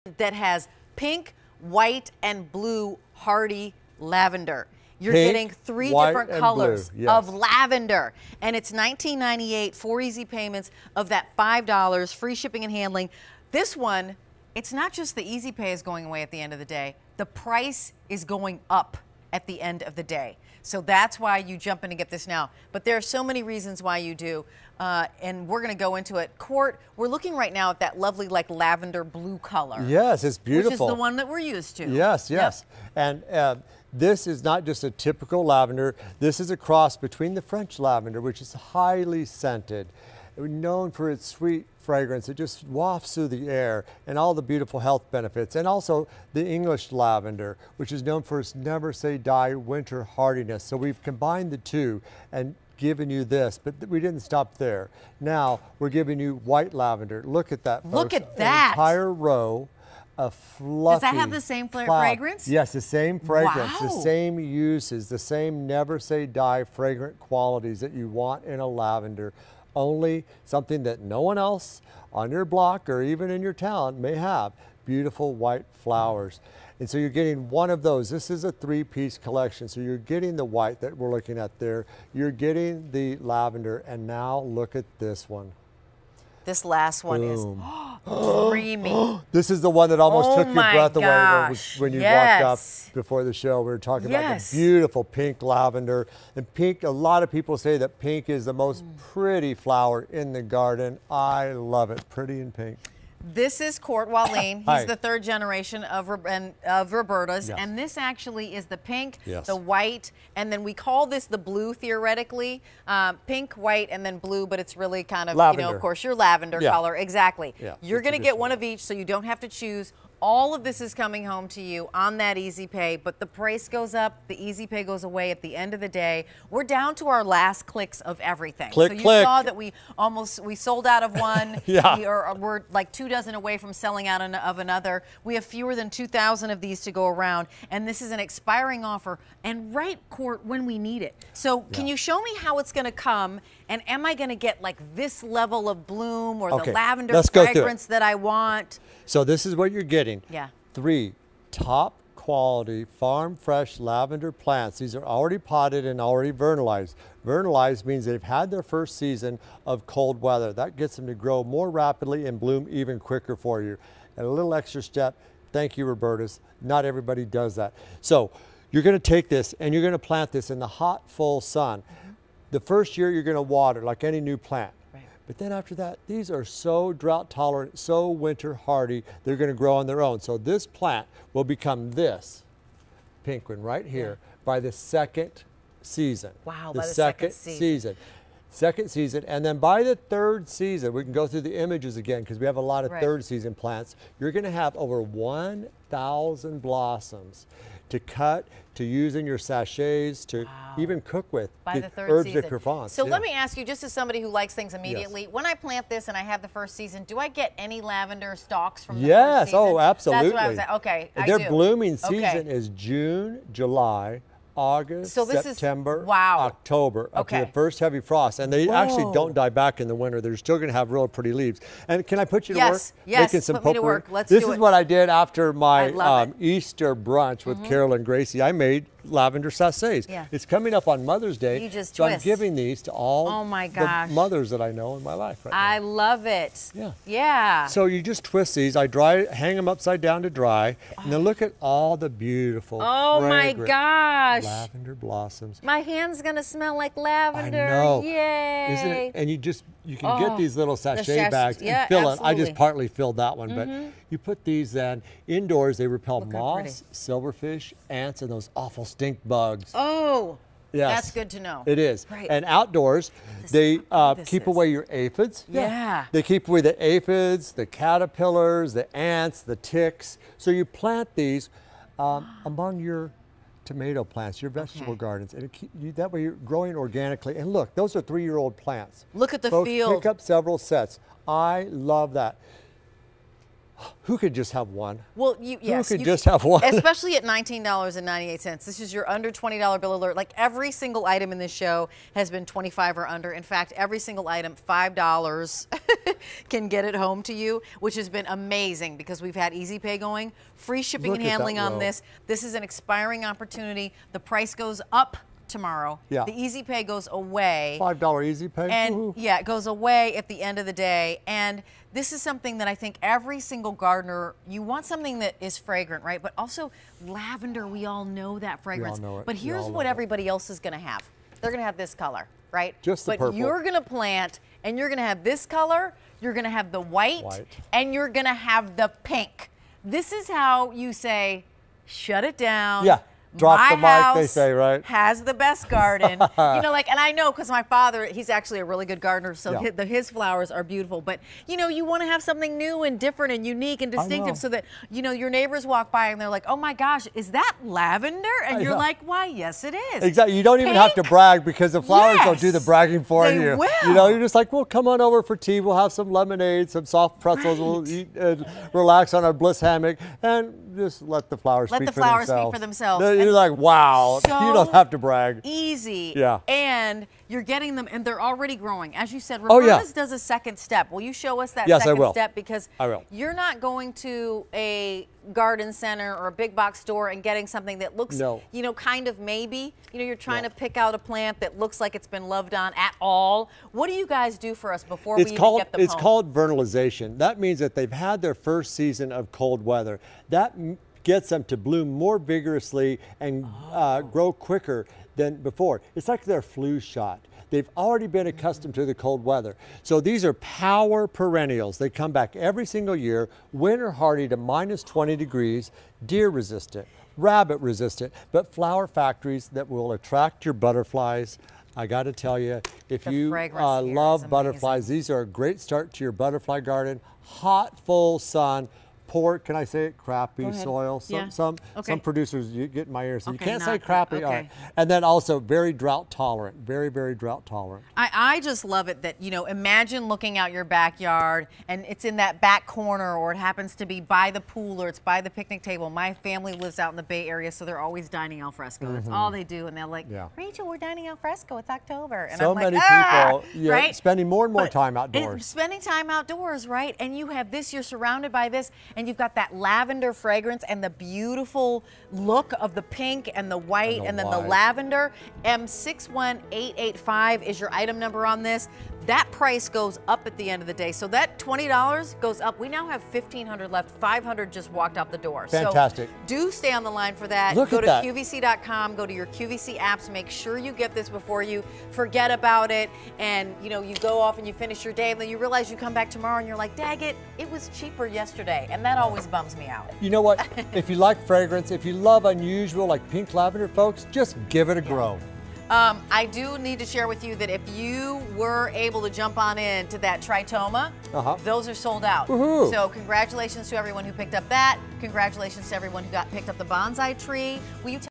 Roberta's 3-Piece Pink, White and Blue Hardy Lavender on QVC
Roberta's 3-Piece Pink, White and Blue Hardy Lavender

A wonderful assortment of pink, white, and blue varieties, these hardy lavender plants make an intoxicating addition to any home. From Roberta's.